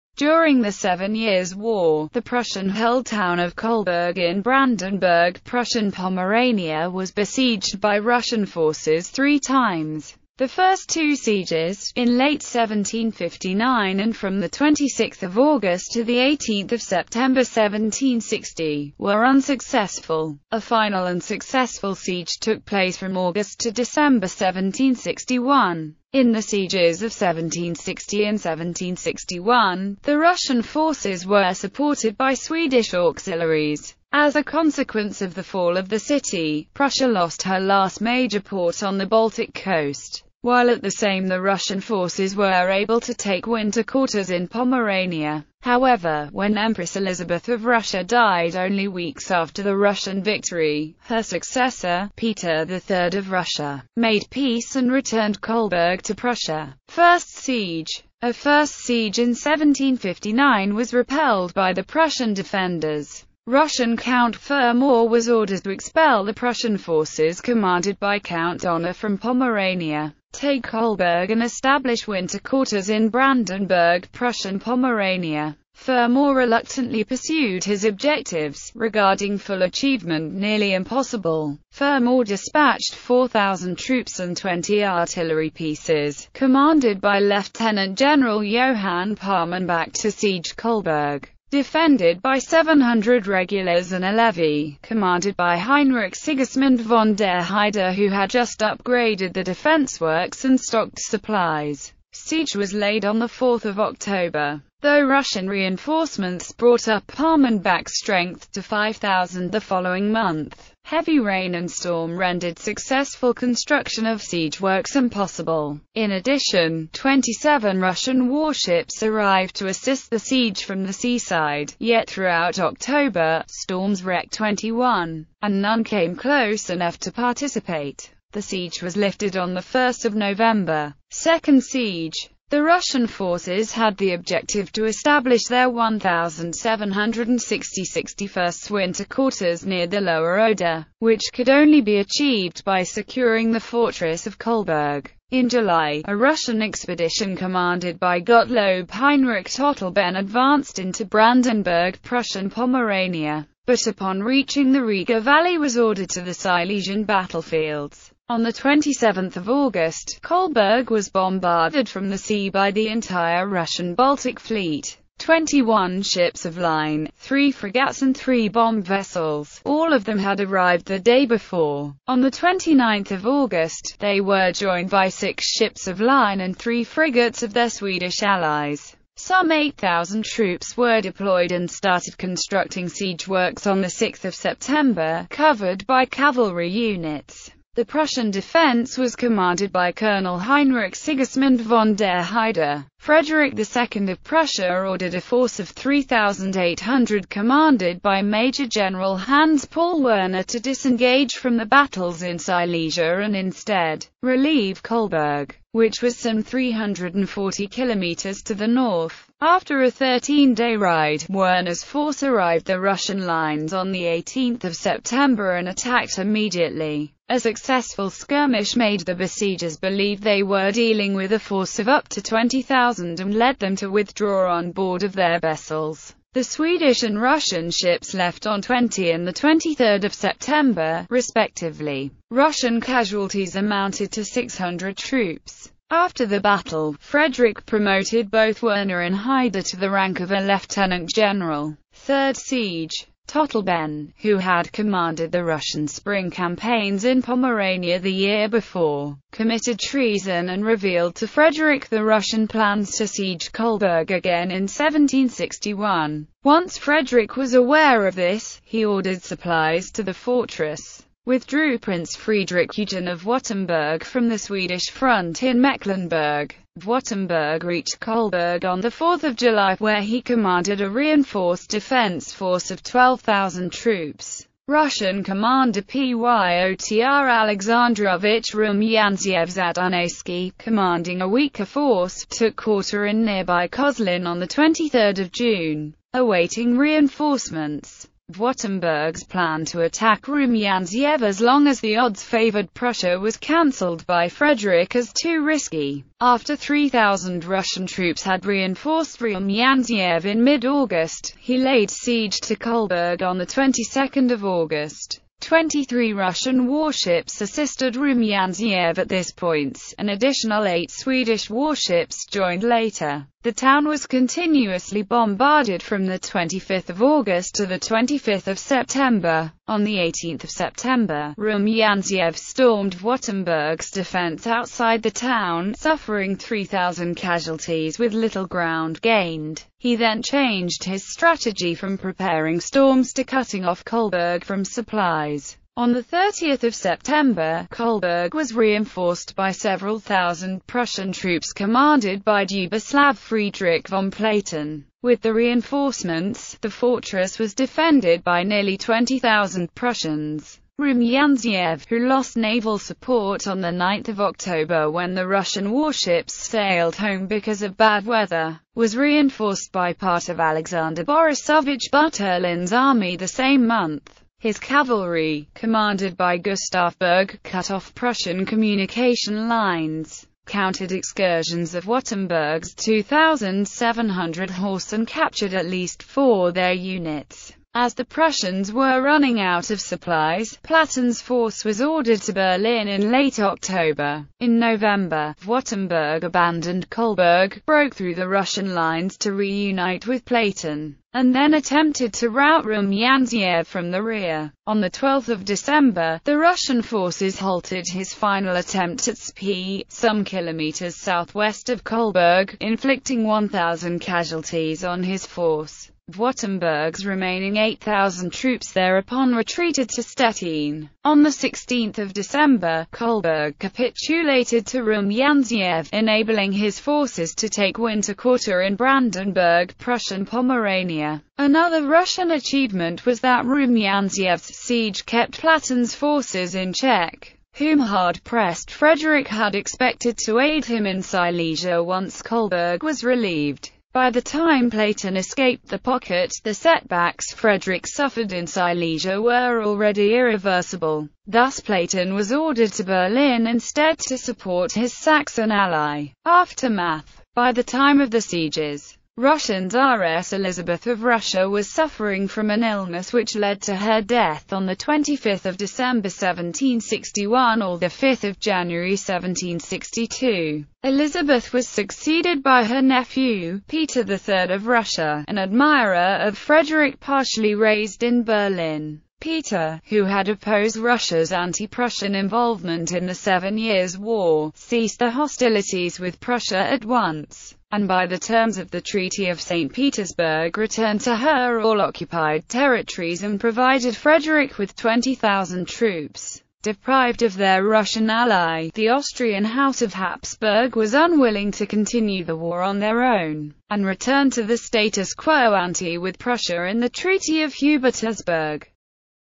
Siege of Kolberg (Seven Years' War)
During the Seven Years' War, the Prussian-held town of Kolberg in Brandenburg-Prussian Pomerania (now Kołobrzeg) was besieged by Russian forces three times.

Image-Copyright-Info
Author-Info:   August_Wilhelm_Pinz_Rauch.jpg: Jotquadrat derivative work: Skäpperöd (talk)

Image-Copyright-Info